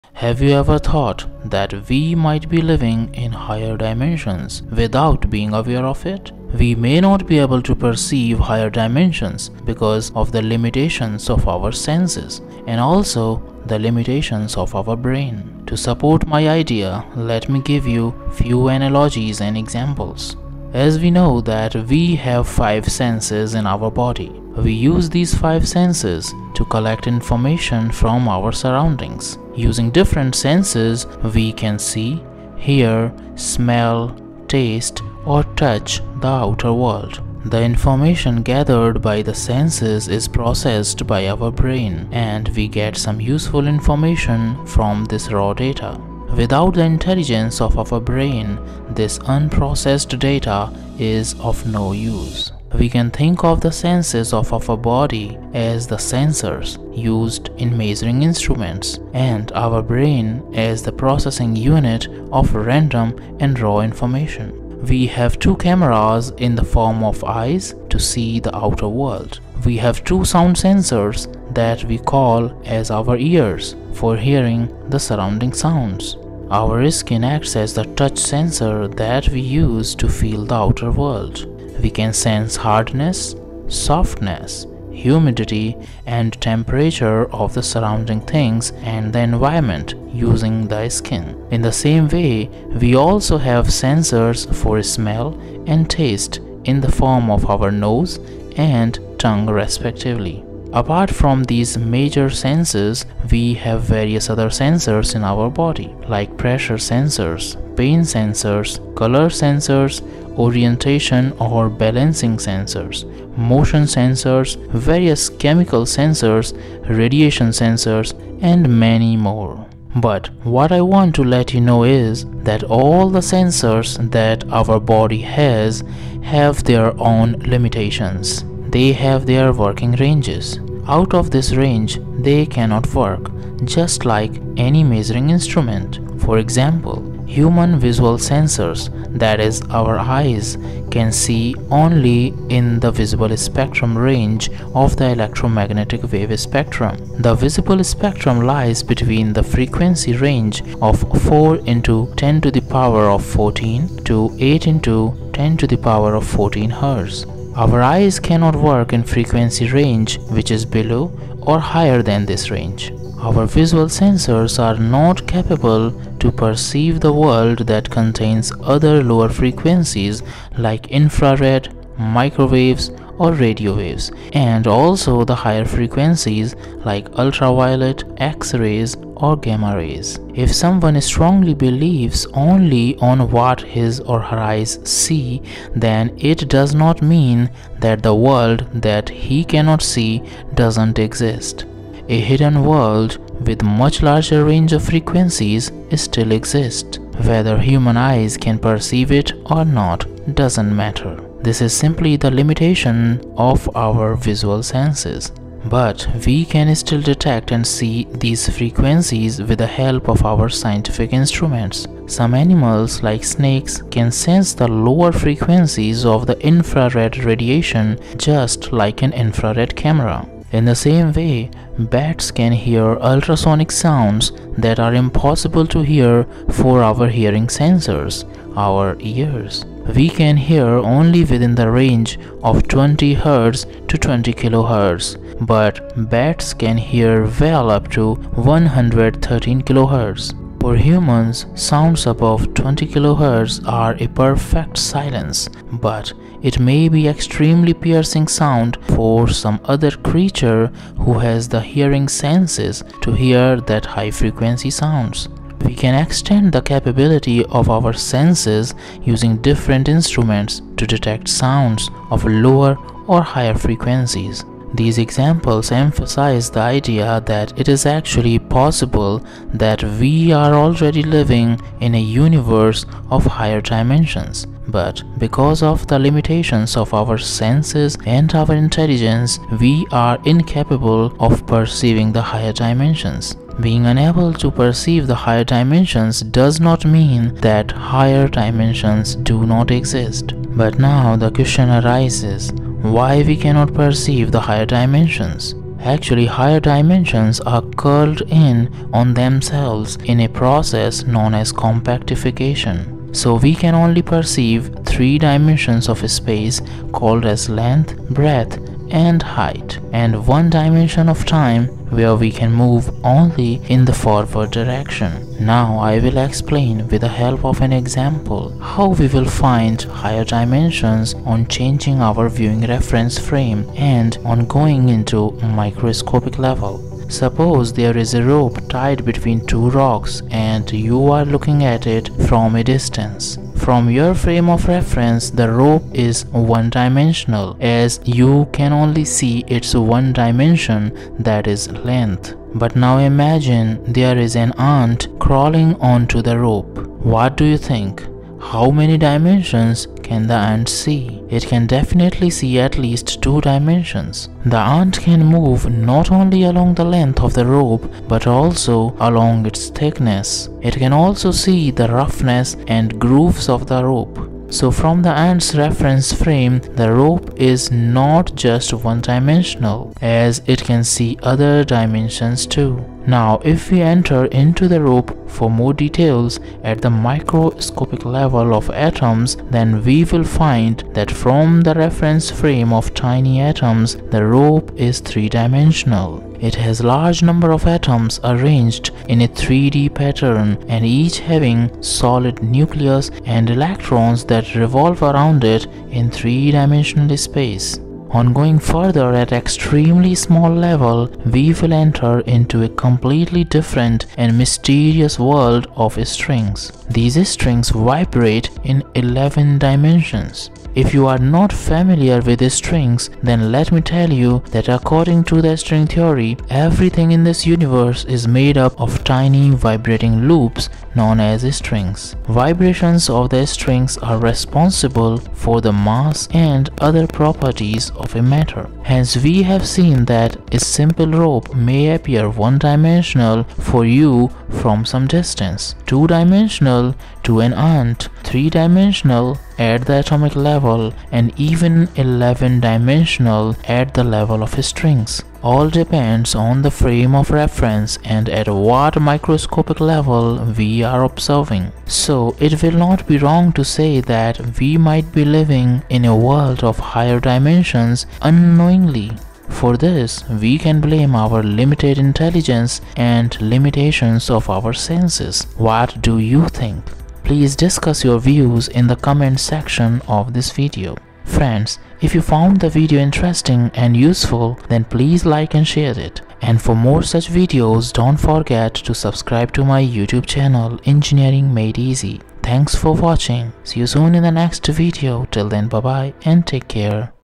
We Might Be Living in Higher Dimensions Unknowingly - Are We Living in Higher Dimensions- Hyperspace
Have you ever thought that we might be living in higher dimensions without being aware of it? So are we really living in higher dimensions or hyperspace? We may not be able to perceive higher dimensions because of the limitations of our senses and also the limitations of our brain.
To support my idea let me give you few analogies and examples...
As we know we have five senses in our body. We use these five senses to collect information from our surroundings. Using different senses we can see, hear, smell, taste or touch the outer world.
The information gathered by the senses is processed by our brain and we get some useful information from this raw data.
You can think of the senses of our body as the sensors used in measuring instruments and our brain as the processing unit of random and raw information.
We have two cameras in the form of eyes to see the outer world, two sound sensors (ears) for hearing the surrounding sounds. Our skin acts as the touch sensor that we use to feel the outer world. We can sense hardness, softness, humidity and temperature of the surrounding things.
In the same way we also have sensors for smell and taste in the form of our nose and tongue respectively.
we have other sensors in our body like pressure sensors, pain sensors, color sensors, orientation or balancing sensors,  motion sensors, various chemical sensors, radiation sensors and many more.
But what I want to let you know is; that all the sensors that our body has; have their own limitations.
They have their working ranges. Out of this range they can not work, just like any measuring instrument.
For example- Human visual sensors that is our eyes, can see only in the visible spectrum range of the Electromagnetic wave spectrum.
Our visual sensors are not capable to perceive the world that contains other lower frequencies like infrared, microwaves or radio waves and also the higher frequencies like ultraviolet, x rays or gamma rays.
If someone strongly believes only on what his or her eyes see, then it does not mean that the world that he can not see, doesn't exist. A hidden world with much larger range of frequencies still exists, whether humans eyes can perceive it or not, doesn't matter. This is simply the limitation of our visual senses.
But we can still detect and see these frequencies with the help of our scientific instruments. Some animals like snakes can sense the lower frequencies of the infrared radiation just like an infrared camera.
In the same way bats can hear ultrasonic sounds that are impossible to hear for our hearing sensors. We can hear only within the range of 20 Hz to 20kHz. But bats can hear well up to 113kHz.
So it may actually be possible that we are already living in a universe of higher dimensions but because of the limitations of our senses and our intelligence, we are incapable of perceiving the higher dimensions.
But why we cannot perceive the higher dimensions?
Actually higher dimensions are curled in on themselves in a process known as compactification.
So we can only perceive three dimensions of space called as length, breadth and height and one dimension of time where we can move only in the forward direction.
Now I will explain with the help of an example, how we will find higher dimensions on changing our viewing reference frame and on going into microscopic level.
Suppose there is a rope tied between two rocks and you are looking at it from a distance. From your frame of reference, the rope is one dimensional (length). But now imagine, there is an ant crawling onto the rope. It can definitely see at least two dimensions. The ant can move not only along the length of the rope but also along its thickness. It can also see the roughness and grooves of the rope. So from the ant's reference frame, the rope is not just one dimensional as it can see other dimensions too.
Now if we enter into the rope for more details, at the Microscopic level of atoms, then we will find that from the reference frame of tiny atoms the rope is three dimensional. It has large number of atoms arranged in a 3D pattern and each having solid nucleus and electrons that revolve around it in 3 dimensional space.
On going further at extremely small level we will enter into the world of strings. These strings vibrate in 11 dimensions. According to the string theory, everything in this universe is made up of tiny vibrating loops known as strings. Vibrations of the strings are responsible for the mass and other properties of a matter.
Hence we have seen that a simple rope may appear one dimensional for you from some distance, two dimensional to an ant, three dimensional at the atomic level and even 11 dimensional at the level of strings. All depends on the frame of reference and at what microscopic level we are observing.
So it will not we wrong to say that we might be living in a world of higher dimensions unknowingly. For this, we can blame our limited intelligence and limitations of our senses.